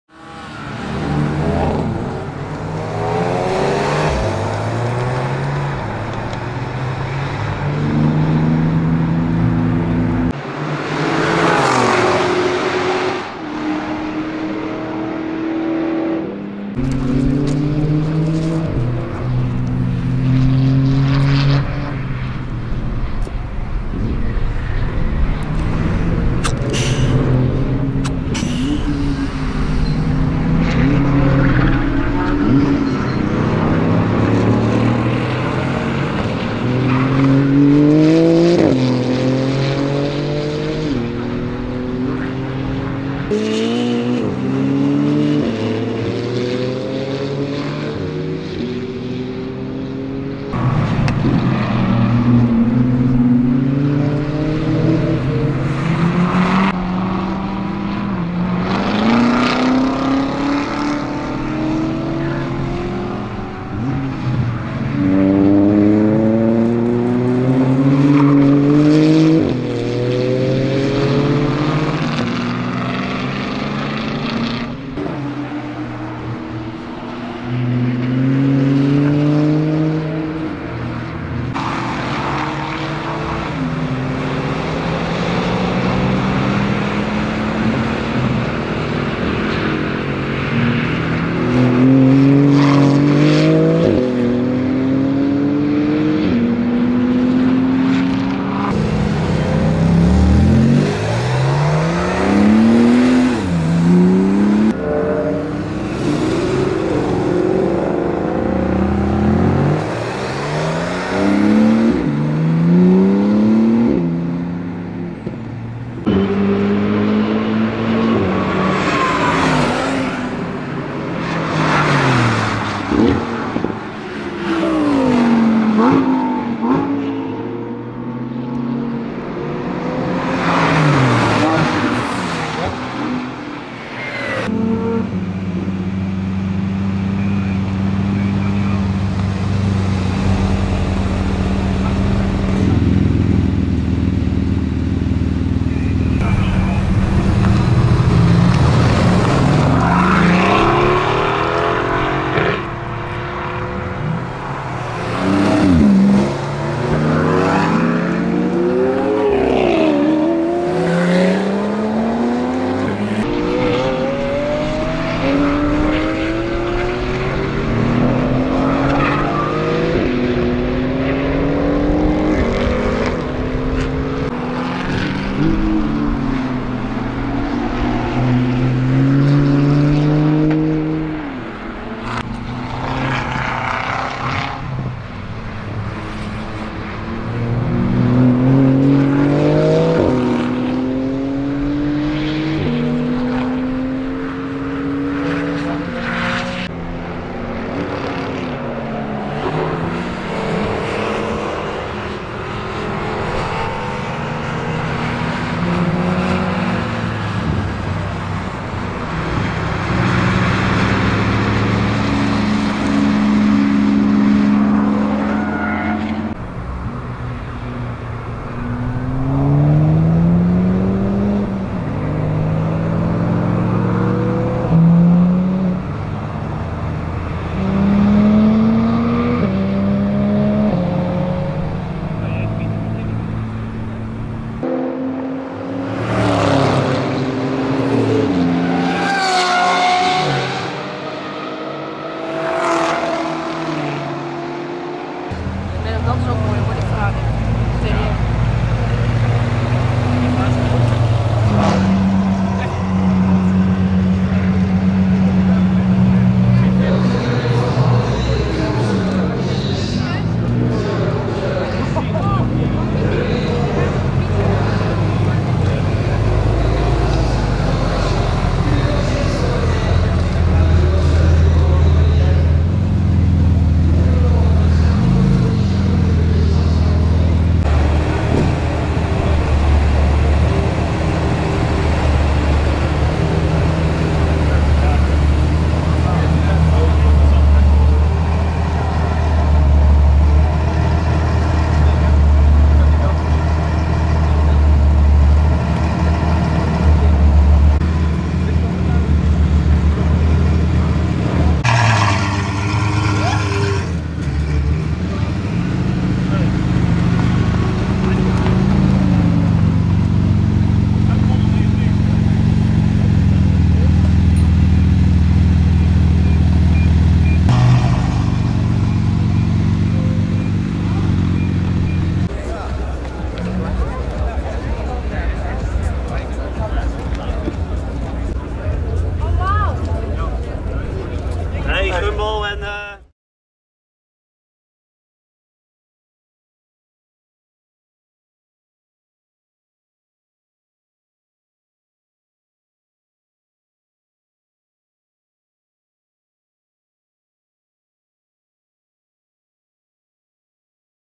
2018 Porsche 991 GT2 RS Going FLATOUT on Track!
This time I have filmed the brand new 2018 Porsche 991 GT2RS going flatout on the track during a supercar gather in The Netherlands. This Porsche is rocket!